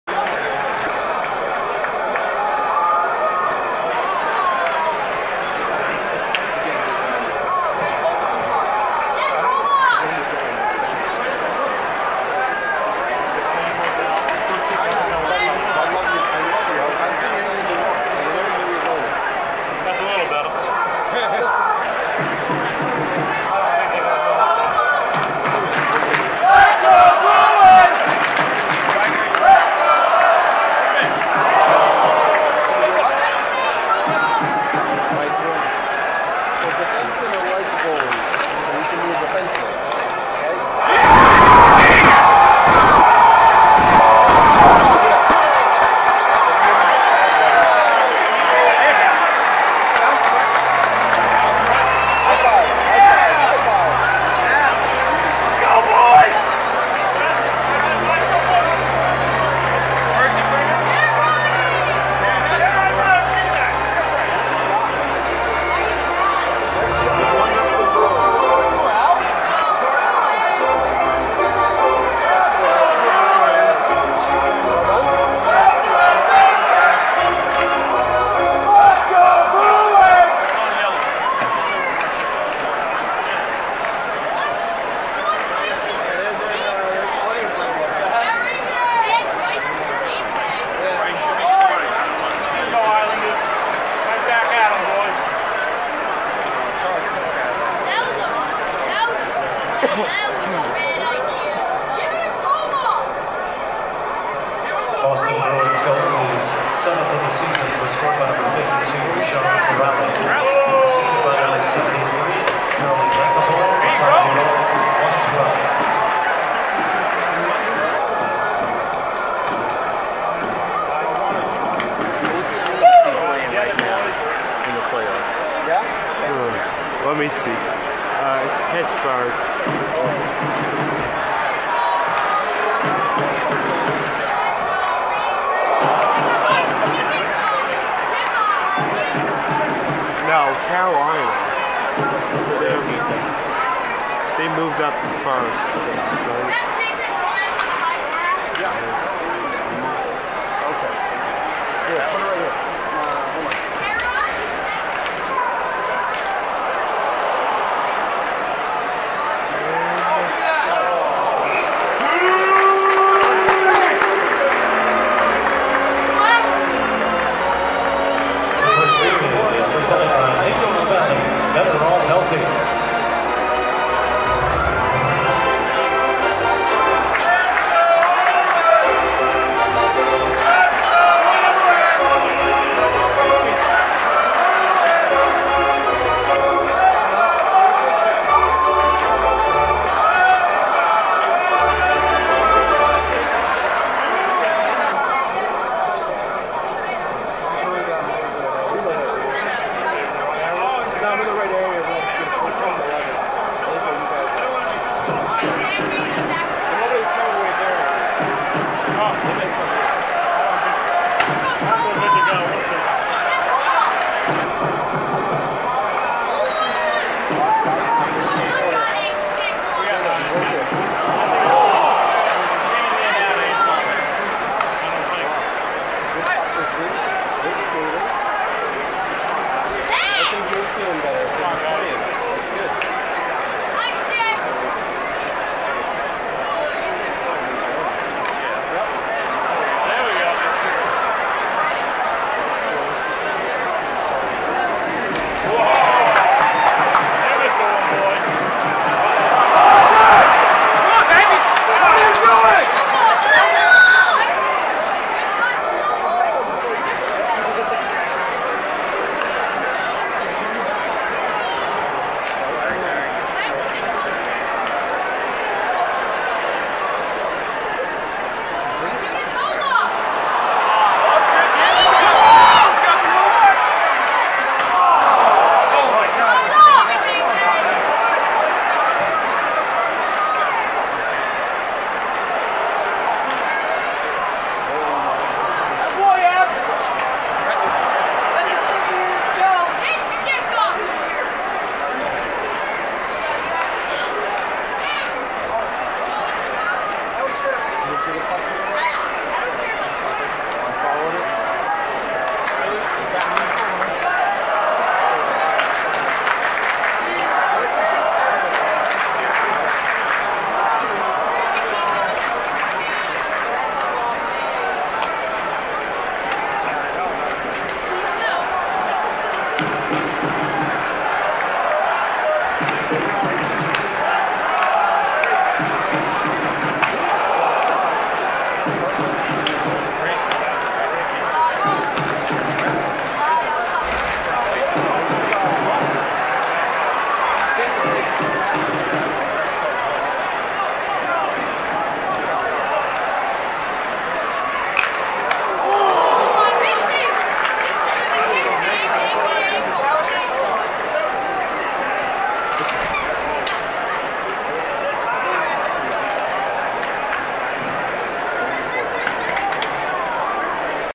My new Nassau Coliseum game,NY Islanders vs Boston Bruins(Part 1)
Here's the start of the first period
Date:Tuesday March 19th 2019